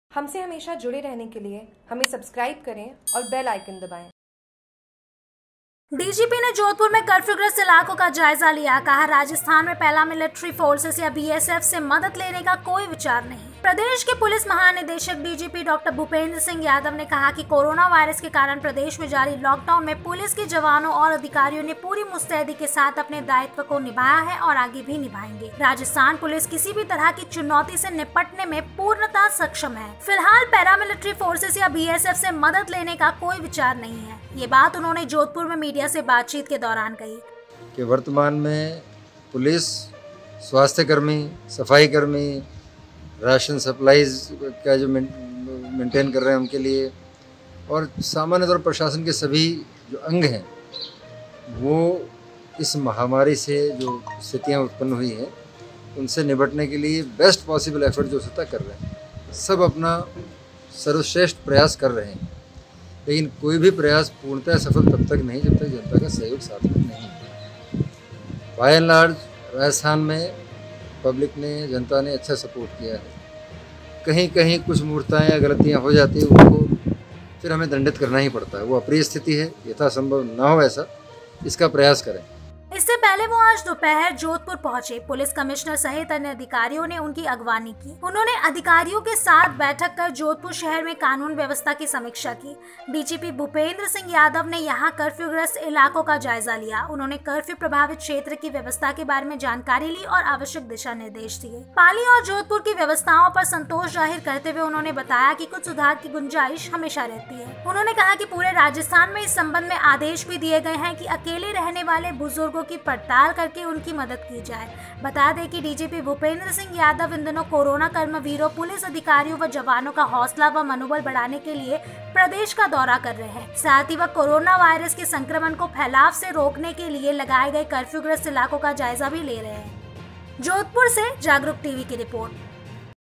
Jagruk TV जोधपुर: डीजीपी ने कर्फ्यूग्रस्त इलाकों का जायजा लिया
Jagruk TV

डीजीपी ने जोधपुर में कर्फ्यूग्रस्त इलाकों का जायजा लिया

कहा-राजस्थान में पैरामिलिट्री फोर्सेज या बीएसएफ से मदद लेने का कोई विचार नहीं

प्रदेश के पुलिस महानिदेशक (डीजीपी) डॉ. भूपेंद्रसिंह यादव ने कहा कि कोरोना वायरस के कारण प्रदेश में जारी लॉकडाउन में पुलिस के जवानों और अधिकारियों ने पूरी मुस्तैदी के साथ अपने दायित्व को निभाया है और आगे भी निभाएंगे। राजस्थान पुलिस किसी भी तरह की चुनौती से निपटने में पूर्णतया सक्षम है। फिलहाल पैरामिलिट्री फोर्सेज या बीएसएफ से मदद लेने का कोई विचार नहीं है। यह बात उन्होंने जोधपुर में मीडिया से बातचीत के दौरान कही।

इससे पहले वह आज दोपहर जोधपुर पहुंचे। पुलिस कमिश्नर सहित अन्य अधिकारियों ने उनकी अगवानी की। उन्होंने अधिकारियों के साथ बैठक कर जोधपुर शहर में कानून व्यवस्था की समीक्षा की। डीजीपी भूपेंद्र यादव ने यहां कर्फ्यूग्रस्त इलाकों का जायजा लिया। उन्होंने कर्फ्यू प्रभावित क्षेत्र की व्यवस्थाओं के बारे में जानकारी ली और आवश्यक दिशा निर्देश दिए।

पाली और जोधपुर की व्यवस्थाओं पर संतोष जाहिर करते हुए उन्होंने बताया कि कुछ सुधार की गुंजाइश हमेशा रहती है। उन्होंने कहा कि पूरे राजस्थान में इस संबंध में आदेश भी दिए गए हैं कि अकेले रहने वाले बुजुर्गों की पड़ताल करके उनकी मदद की जाए।

बता दे कि डीजीपी भूपेंद्र सिंह यादव इन दिनों कोरोना कर्मवीरों पुलिस अधिकारियों व जवानों का हौसला व मनोबल बढ़ाने के लिए प्रदेश का दौरा कर रहे है। साथ ही वह कोरोना वायरस के संक्रमण को फैलाव से रोकने के लिए लगाए गए कफ्र्यूग्रस्त इलाकों का जायजा भी ले रहे है